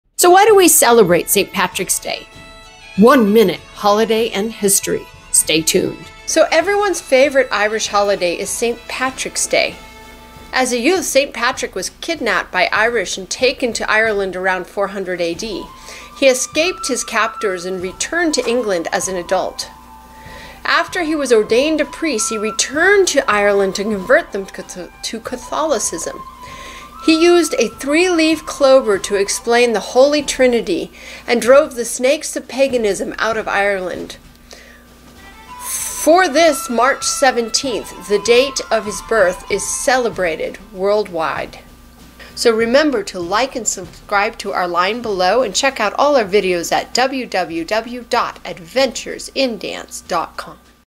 St. Patrick: From Captive to Priest
Discover the captivating story of St. Patrick's Day and the man behind the celebration, from being kidnapped by Irish raiders to his mission of spreading Catholicism in Ireland. Unravel the mystery behind the three-leaf clover symbolism used by St. Patrick as a powerful tool for his teachings.